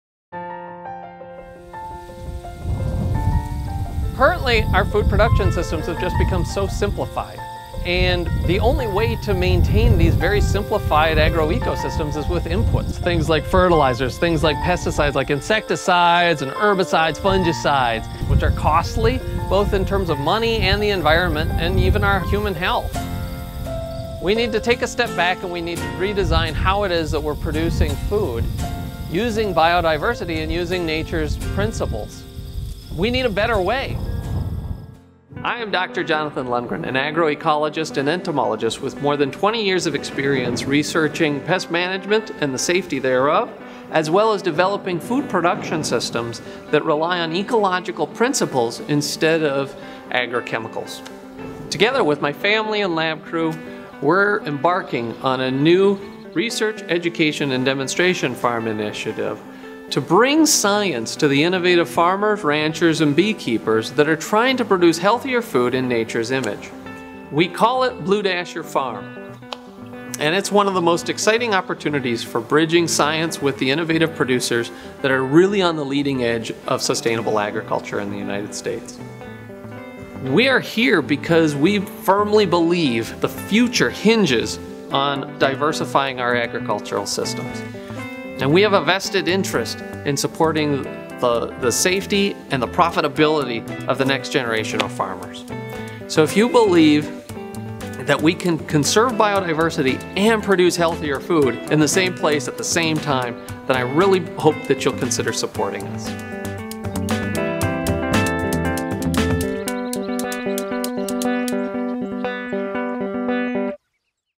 Blue Dasher Farm
Blue Dasher Farm, based in Deuel County South Dakota, is the first in a network of research/education/demonstration farms that will help usher in a paradigm shift in how we produce food.
Credits:  Filmed by Peter Byck and Cable Hardin.  Edited by Cable Hardin.